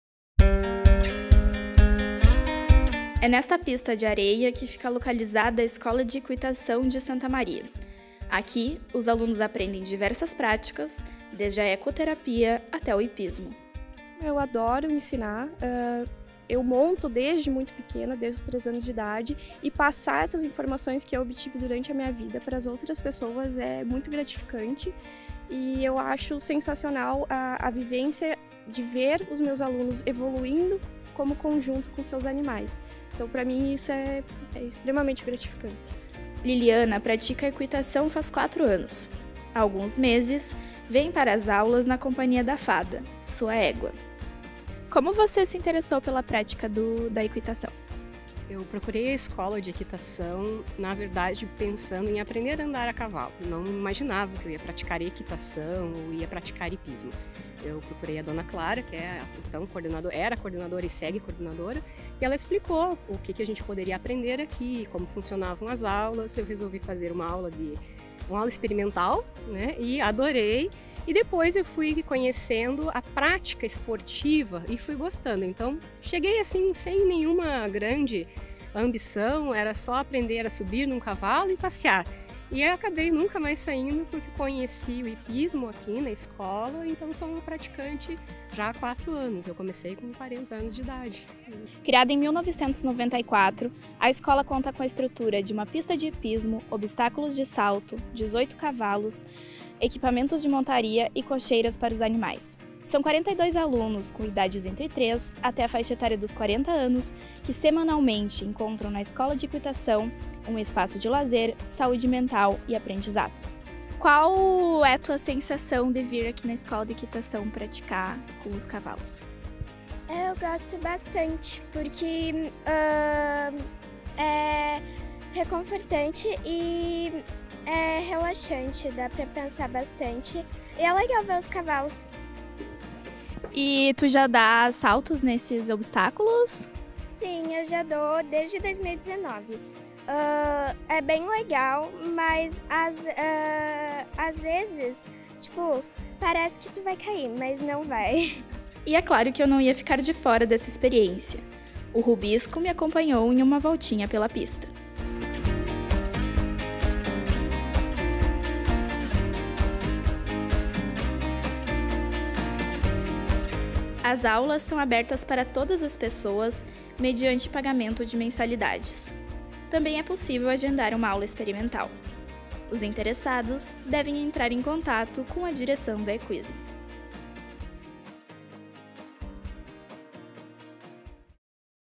Escola de Equitação de Santa Maria promove equoterapia e hipismo
🐴🐎 Diversas atividades de lazer, saúde mental e aprendizado de equitação são promovidas na Escola de Equitação Santa Maria.
📹 Nossa equipe foi conferir na prática uma das aulas promovidas pelas escola. Confira!